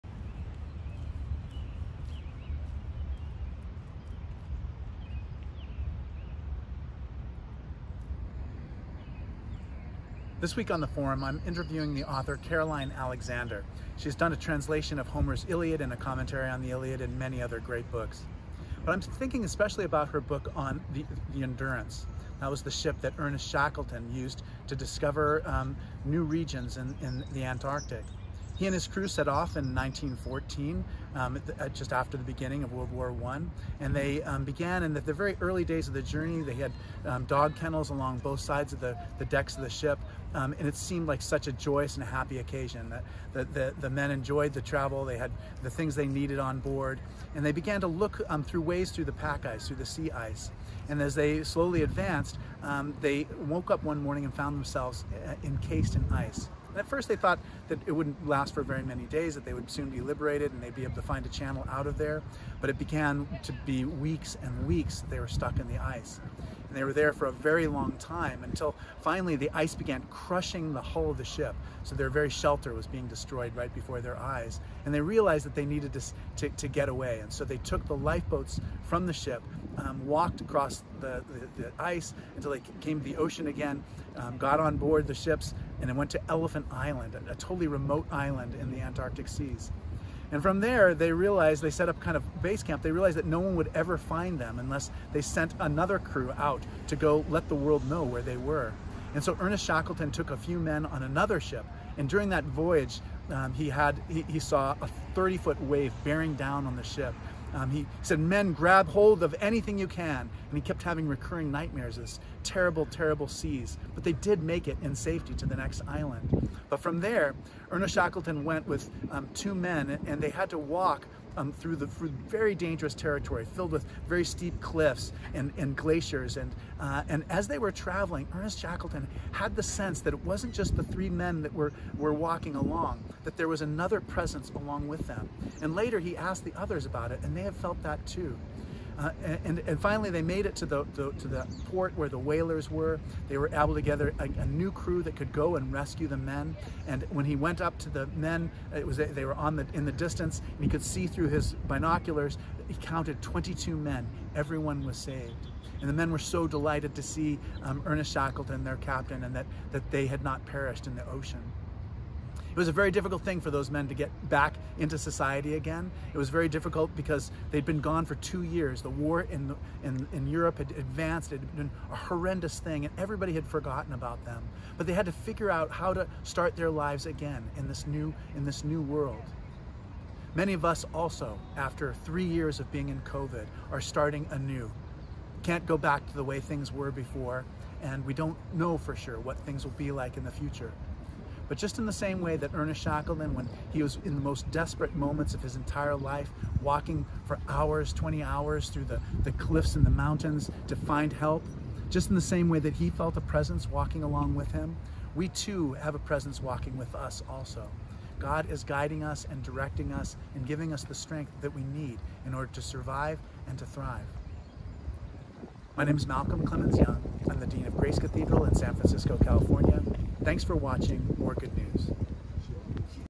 The Ernest Shackletons of Today
The World Health Organization has declared that the Pandemic is over. How do we learn from the Antarctic explorers about how to navigate a world of change?